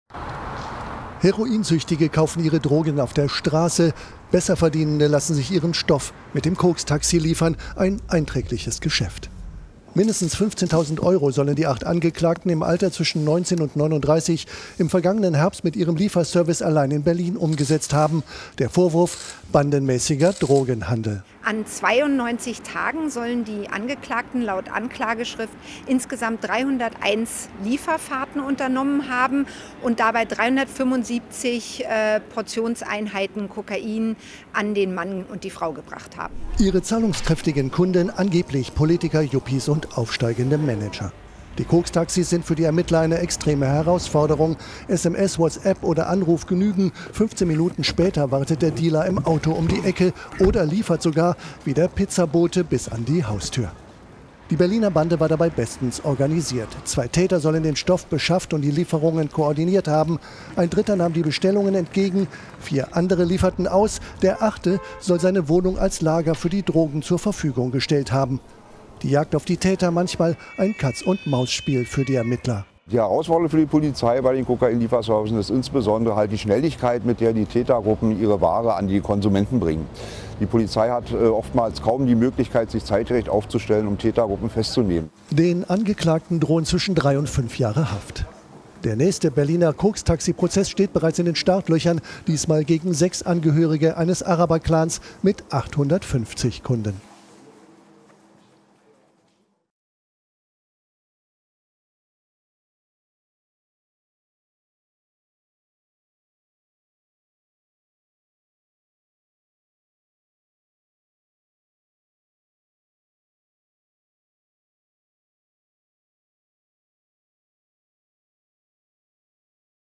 KOKS-TAXIS: Erster Prozess gegen Berliner Drogen-Lieferservice hat begonnen
Unter massiven Sicherheitsvorkehrungen hat in Berlin ein Prozess gegen acht Angeklagte begonnen, die einen professionellen Kokain-Lieferservice betrieben haben sollen. Zu Beginn am Freitag wurde zunächst ein Antrag auf Nichtverlesen der Anklage zurückgewiesen. Selbst auf einem benachbarten Gebäudedach waren Polizisten postiert. Bei den Einlasskontrollen trugen Justiz- und Polizeibeamte schusssichere Westen.

Wegen der Corona-Pandemie läuft die Verhandlung gegen die 19- bis 39-Jährigen nicht im Kriminalgericht, sondern im Veranstaltungszentrum Urania Berlin. Bei 8 Angeklagten und 16 Verteidigern konnte laut Gericht das gesetzlich angeordnete Abstandsgebot in den Sälen des Moabiter Kriminalgerichts nicht eingehalten werden.

Laut Anklage sollen die Männer zwischen Ende Juli und Mitte November 2019 an 92 Tagen bei 301 Lieferfahrten fast 375 Portionen Kokaingemisch verkauft haben. Sie sollen knapp 15 000 Euro eingenommen haben.

Die Bestellungen seien Ermittlungen zufolge telefonisch aufgenommen und per Pkw ausgeliefert worden. Einige der Fahrer hatten den Angaben zufolge keinen Führerschein. Als Kopf der mutmaßlichen Bande gilt ein 39-Jähriger, der die Geschäfte aus einer Bar heraus geleitet haben soll. Dabei habe ihn ein 30-jähriger Angeklagter unterstützt. 30 Prozesstage sind geplant.